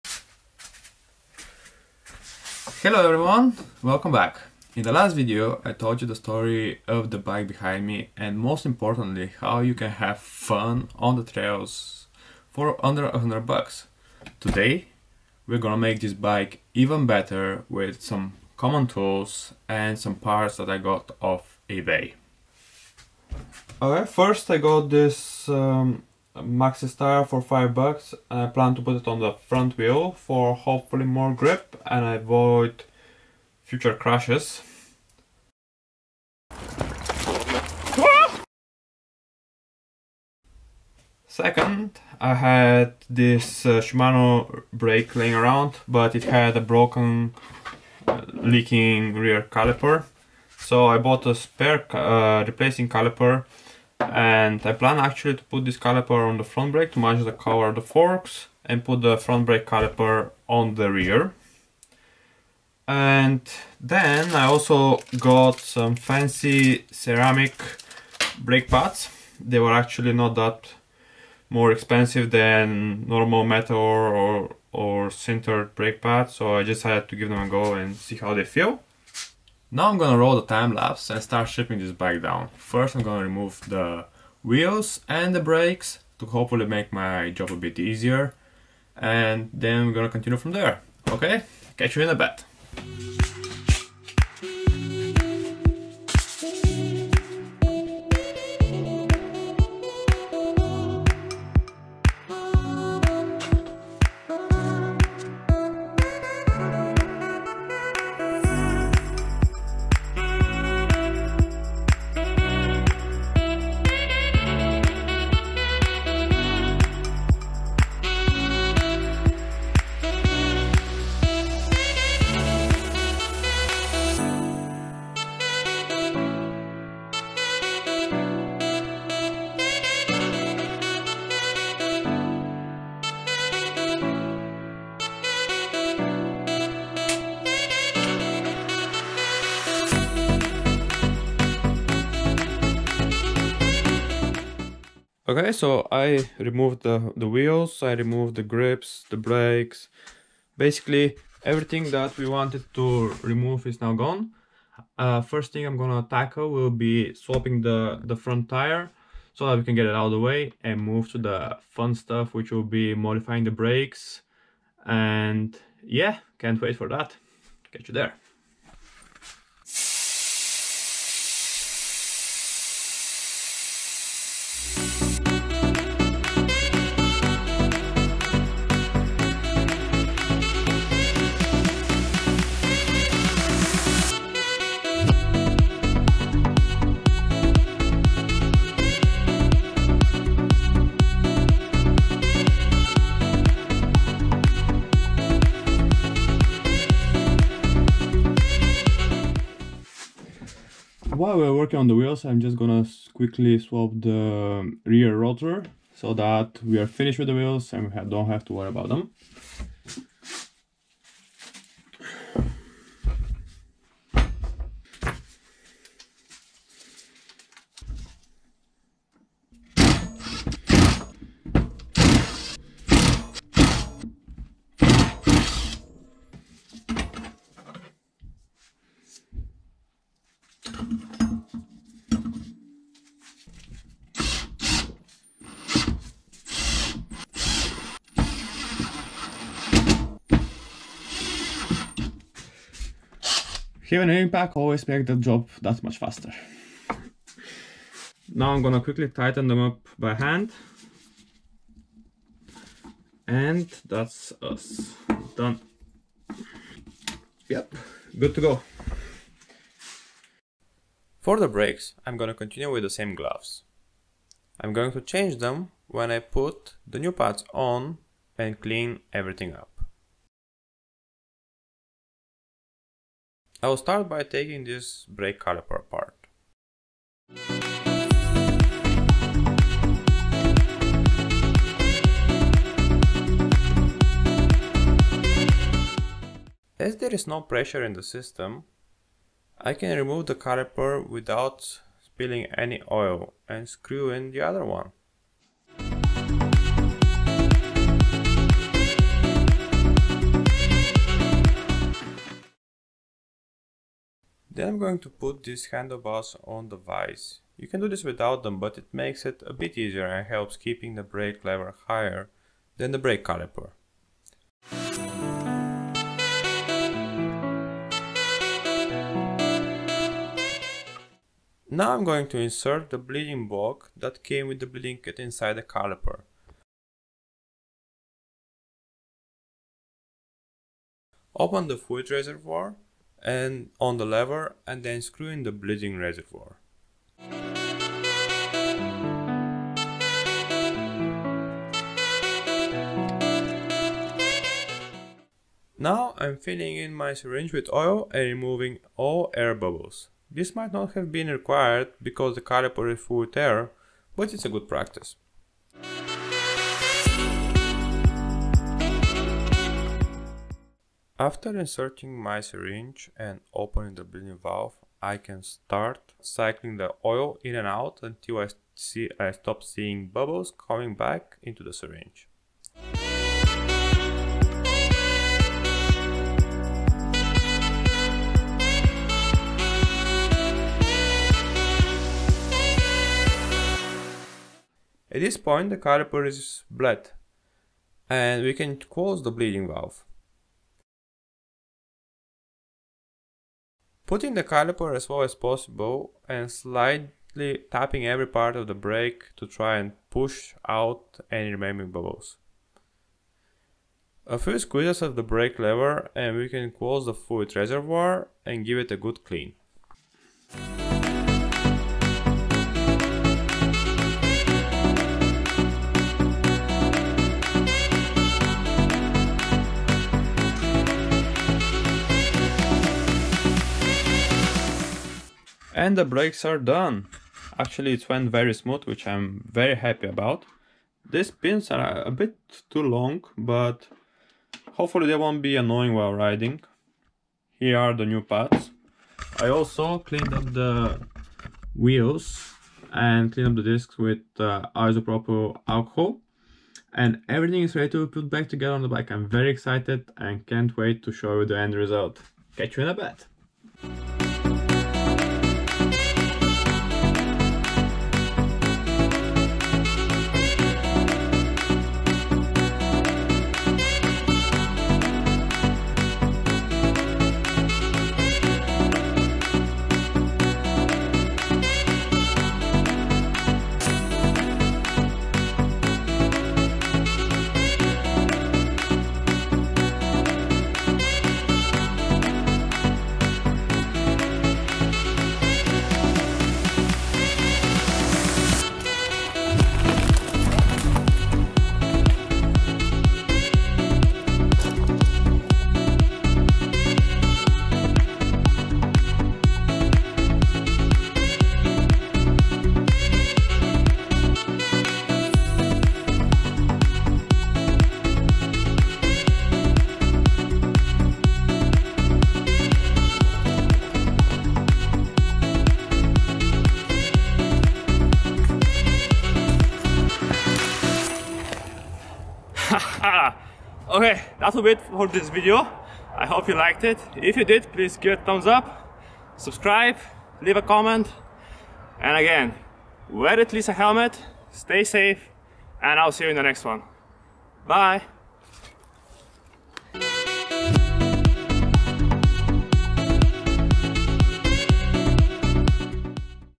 Upgrading my Cheap Hardtail Mountain Bike with Cheap Parts Off Ebay
In this video, I fixed the main issues with my hardtail mtb: front tyre and brakes. I got a beefier maxxis highroller tyre and fixed an old Shimano hydraulic brake that had a leaking caliper. As part of this, I also upgraded the brake pads to ceramic pads and they feel fantastic. Now the bike feels great and has a lot of grip.

I have a few other projects that I plan to finish in the upcoming videos so let me what do you want me to cover.